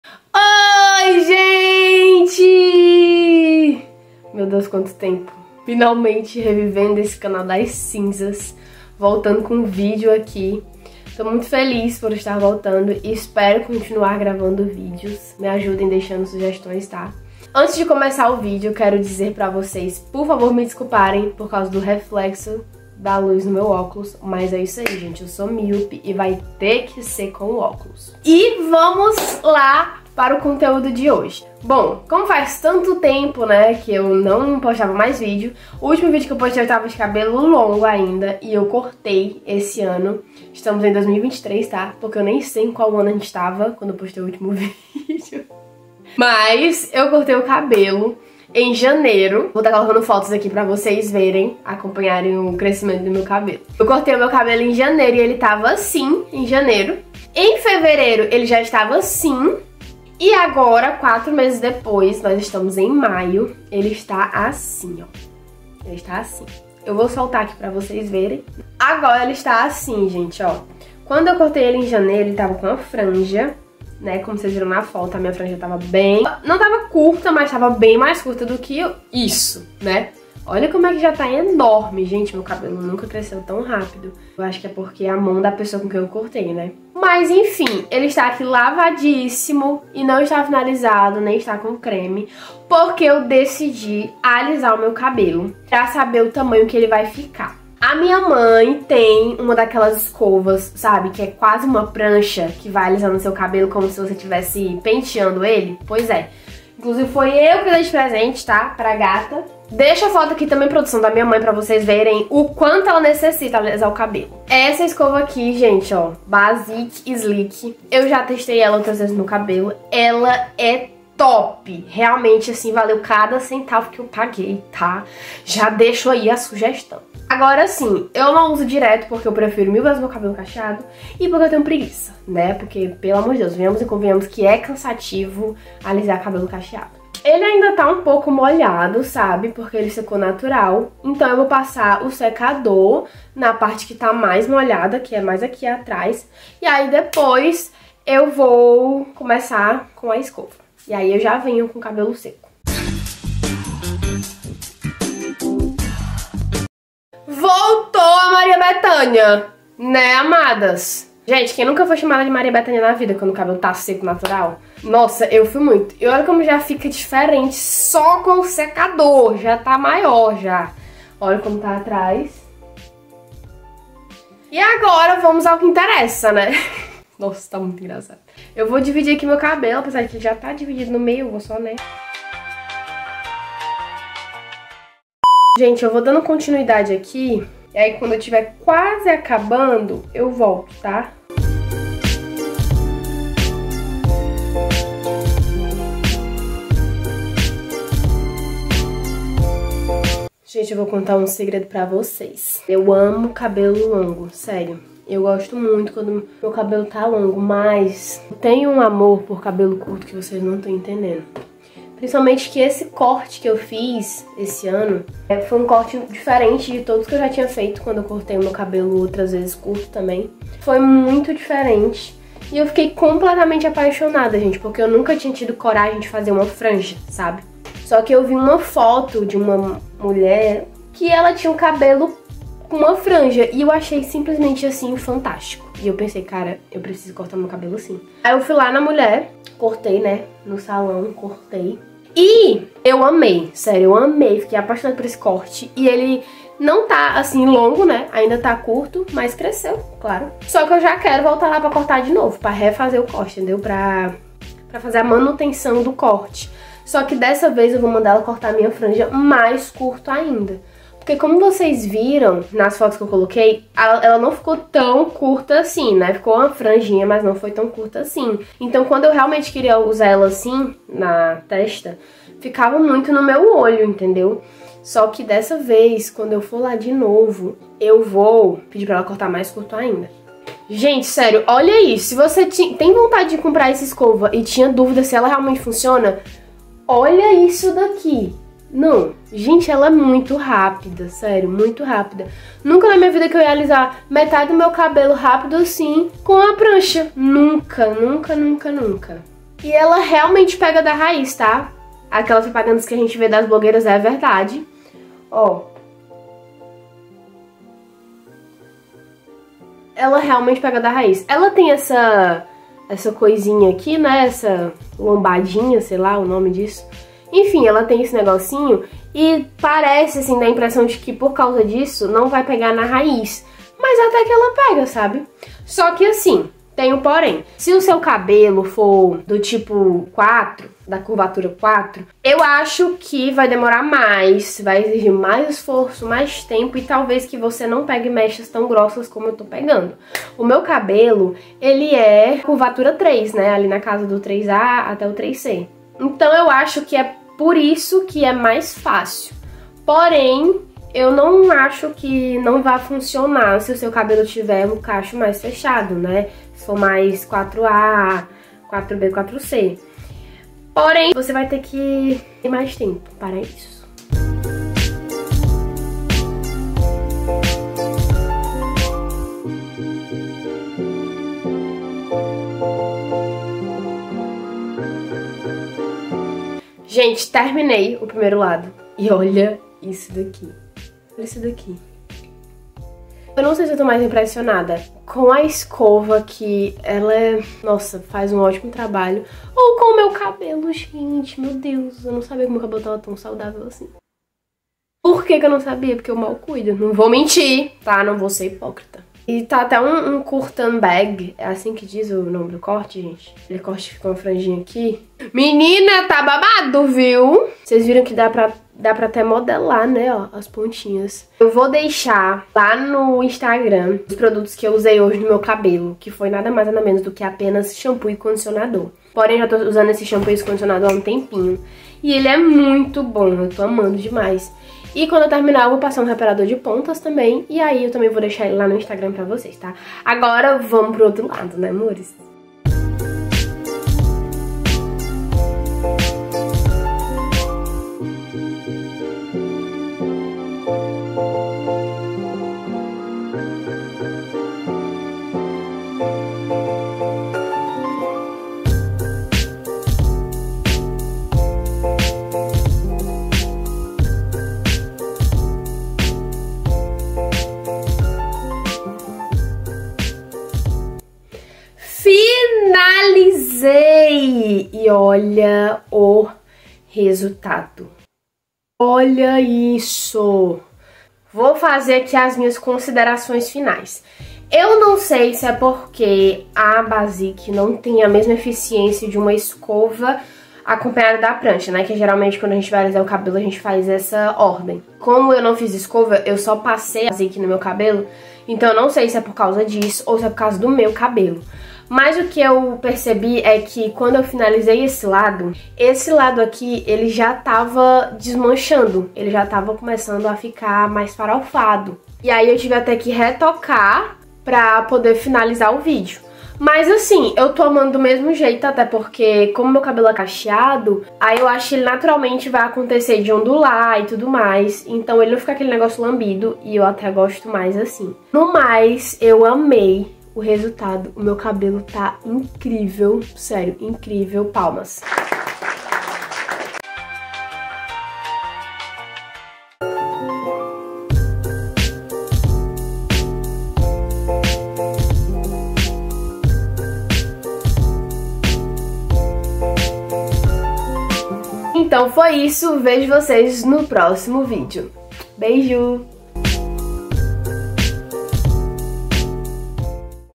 Usei a escova Basiqe Sleek no meu cabelo CACHEADO!!!!
Oiii, gente! No vídeo de hoje eu usaria a escova alisadora Basiqe Sleek no meu cabelo para saber como ia ficar.

Se você quer saber o resultado, acompanha o vídeo até o final! 😘